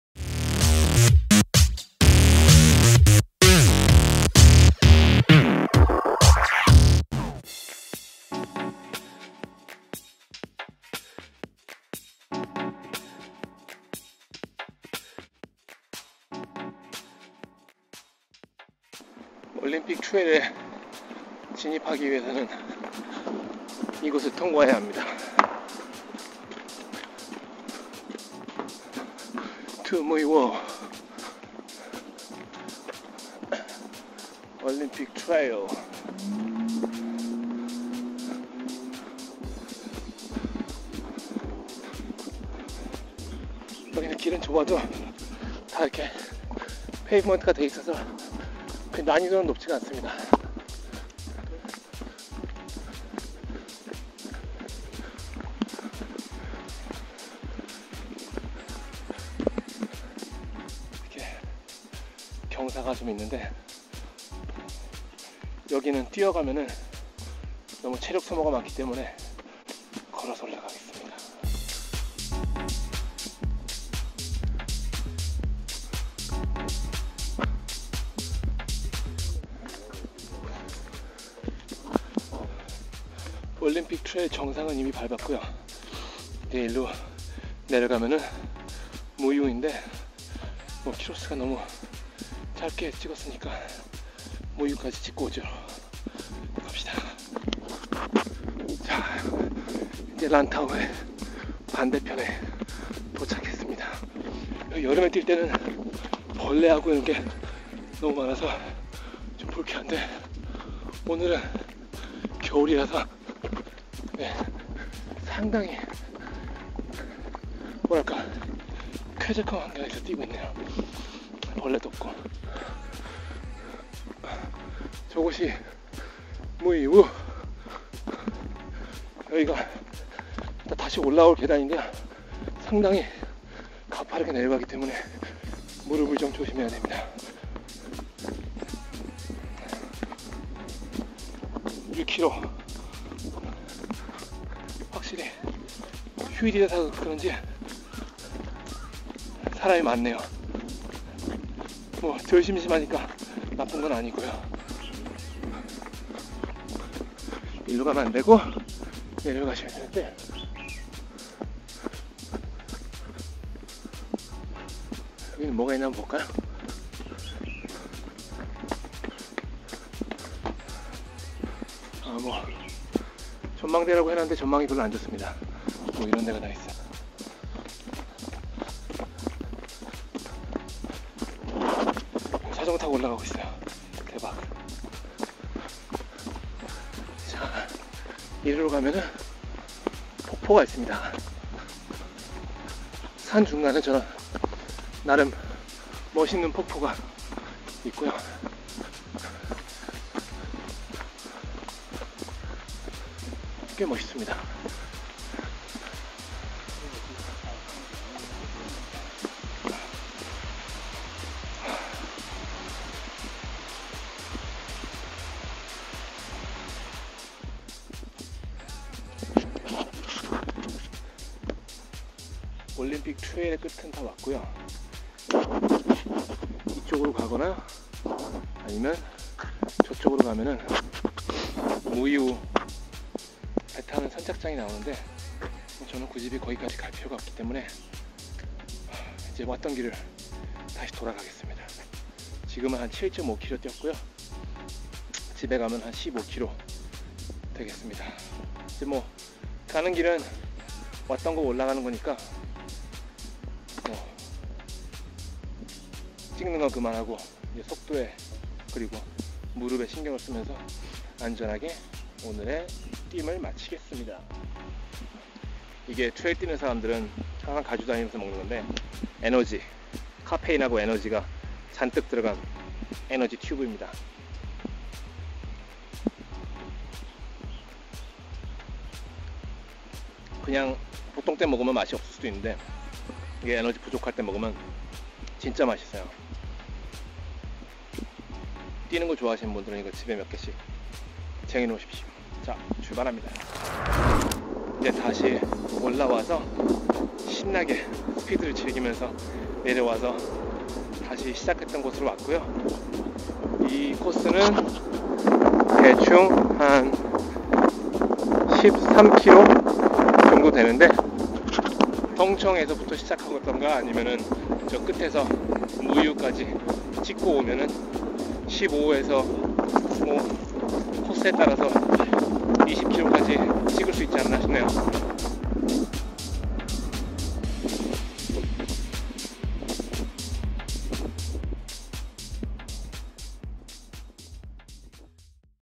홍콩 트레일 러닝 시리즈 - 올림픽 트레일 (란타우섬) 편
색다른 홍콩 여행을 원하시는 여행자분들과 현지분들께 제가 다니며 얻은 트레일러닝/하이킹 정보를 틈틈이 공유하려합니다.

란타우 올림픽 트레일에 대한 더 자세한 정보를 원하시면 다음 링크를 참조해주시기 바랍니다